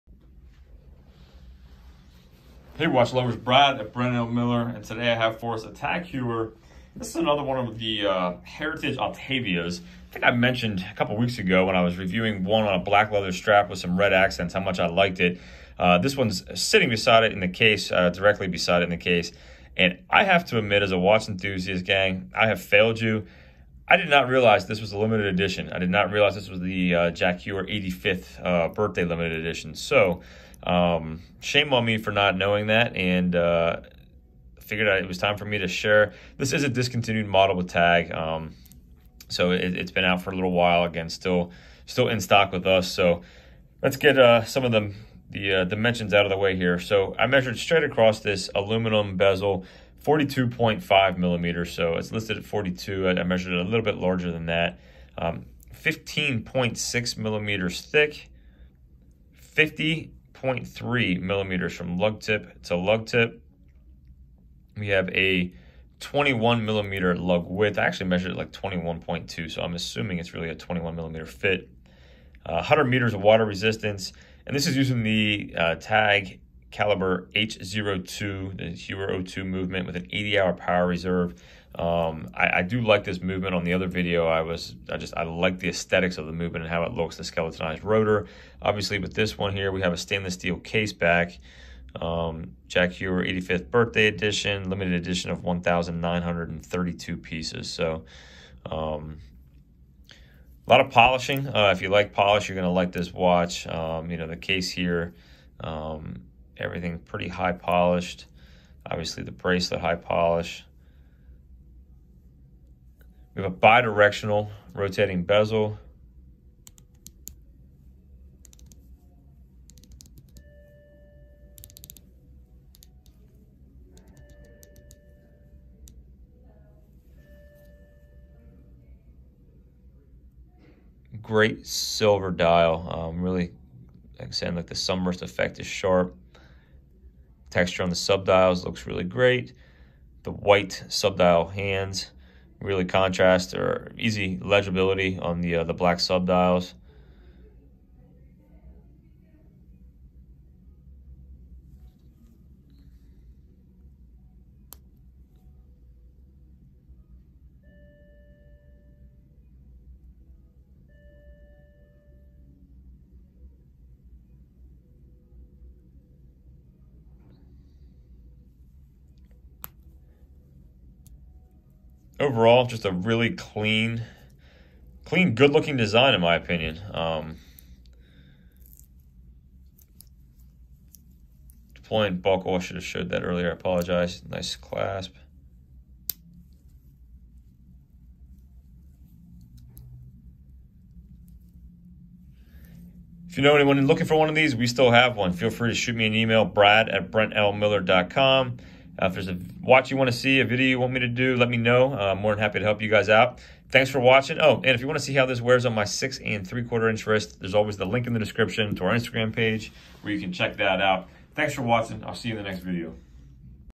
Tag Heuer Heritage Autavia (Limited Edition)
Jack Heuer 85th LE
CBE2111.BA0687
42.5mm case
15.6mm thick
50.3mm lug to lug
21mm lug width
100M water resistance
Cal. Heuer 02 w/ 80 hours power reserve